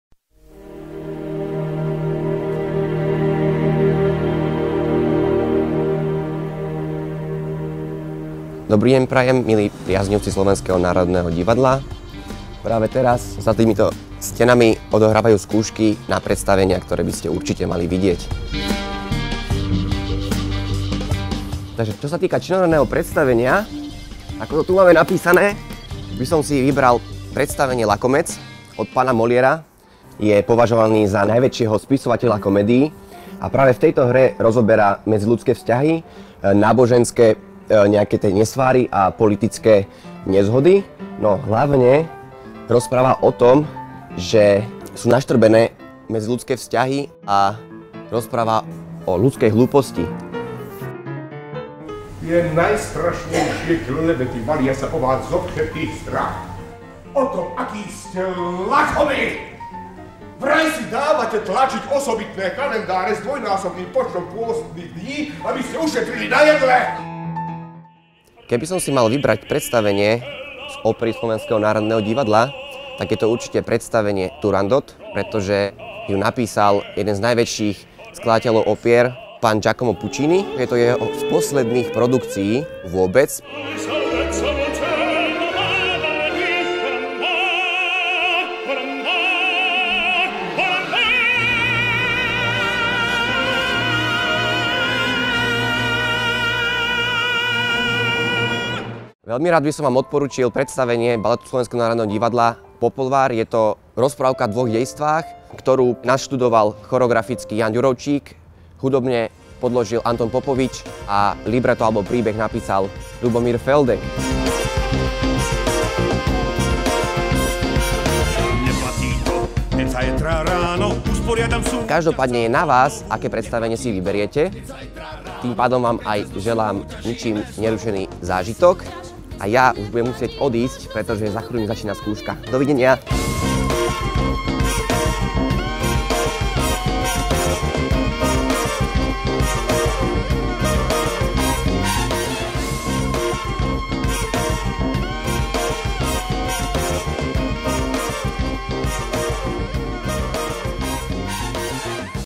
Peter Dedinský pozýva do SND
Sólista Baletu SND Peter Dedinský vstúpil na našu prvú scénu pred siedmimi rokmi a za ten čas vystupoval aj na javiskách v Kanade, Francúzsku, Rakúsku, Českej republike, či Maďarsku. Študoval klasický tanec na Tanečnom konzervatóriu Evy Jaczovej v Bratislave a zároveň sa zúčastnil tanečnej stáže Ballet master classes v Prahe. Na svojom konte má hneď dve ceny Literárneho fondu.